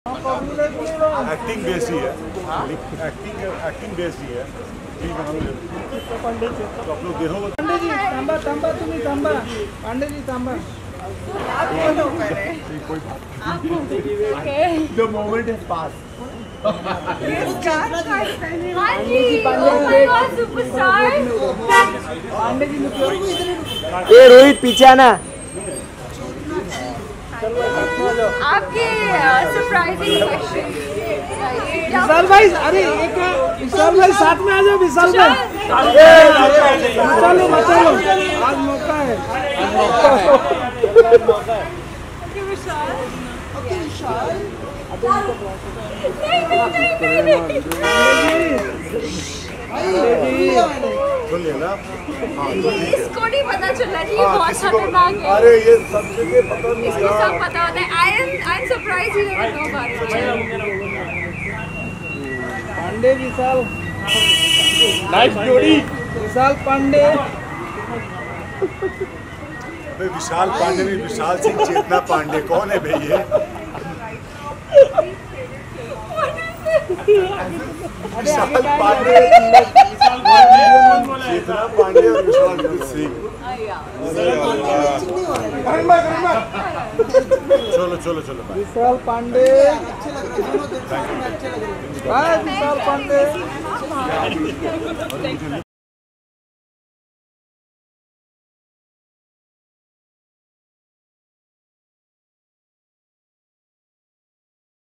Vishal Aditya ने Media के सामने Chetna Pandey को किया Kiss !
India Trending News (ITN) : Vishal Aditya Singh Kissed Chetna Pandey | Vishal Aditya Singh And Chetna Pandey Cute Moment At Night Club After Party